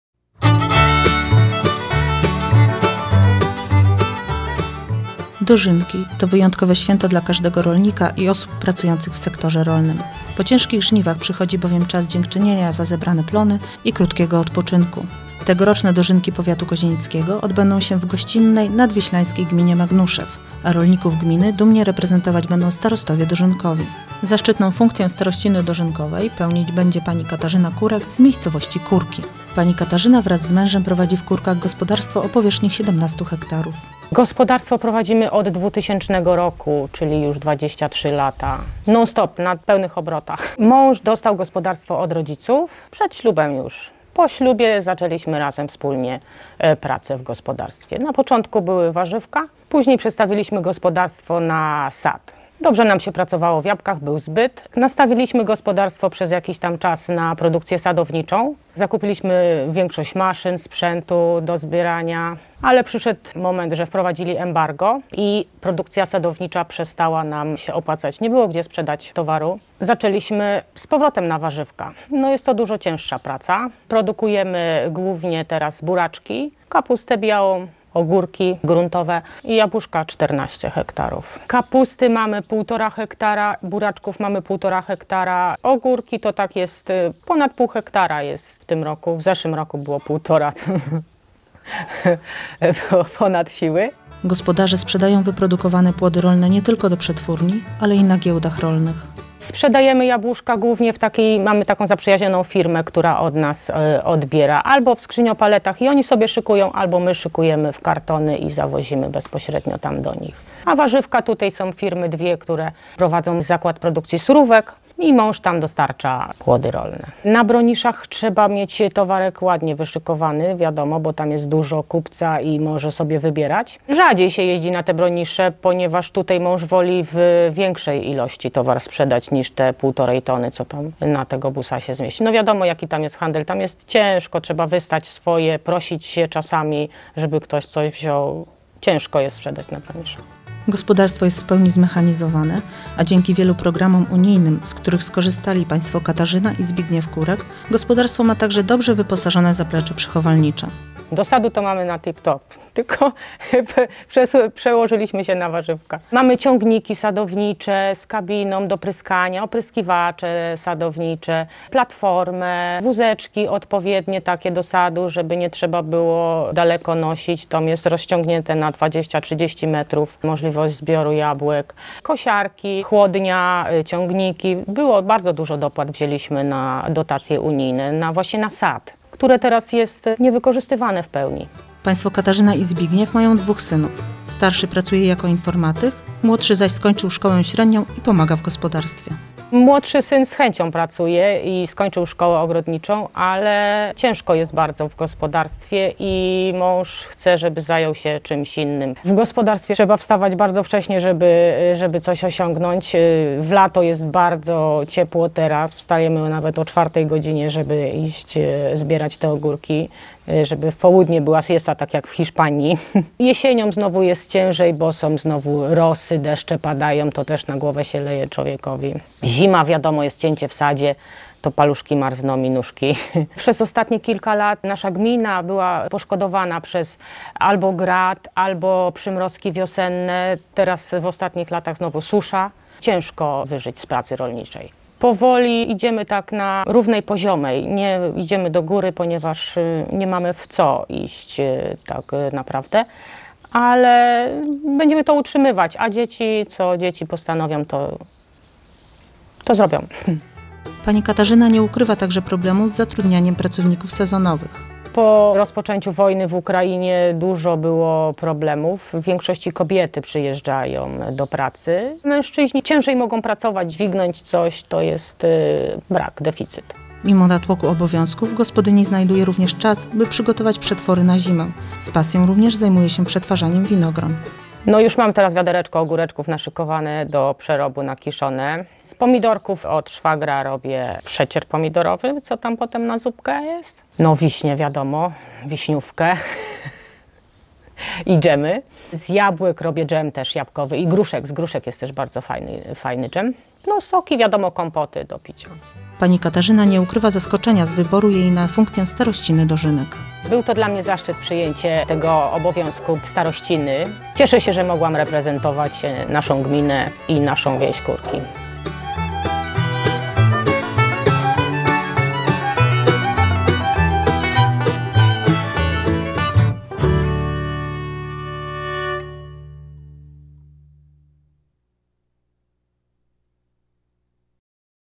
Starościna Dożynek Powiatowych w Magnuszewie (TVNP 2023.07.27)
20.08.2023 roku w Magnuszewie odbędą się XIX Dożynki Powiatu Kozienickiego. Funkcję Starościny Dożynek pełnić będzie Pani Katarzyna Kurek ze wsi Kurki.